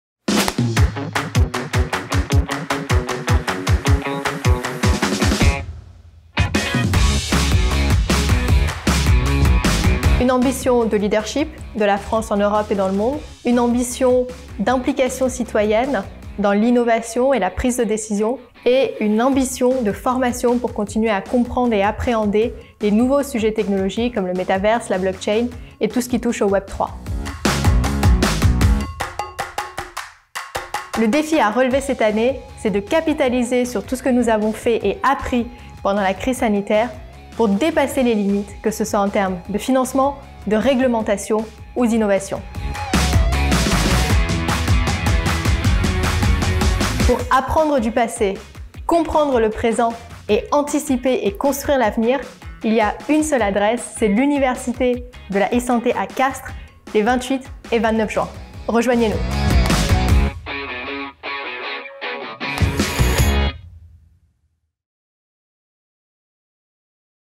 Anca Petre, membre du comité scientifique de l'Université de la e-Santé, répond à 3 questions
Anca Petre, pharmacienne, Digital health & Blockchain COO, 23 consulting.
Membre du comité scientifique de l'Université de la e-santé répond à nos questions :
1 - Quelle(s) ambition(s) pour la e-santé ?
2 - Quel(s) défis à relever en 2022 / 2023 ?
3 - Pourquoi faut-il être à Castres les 28 et 29 juins prochains ?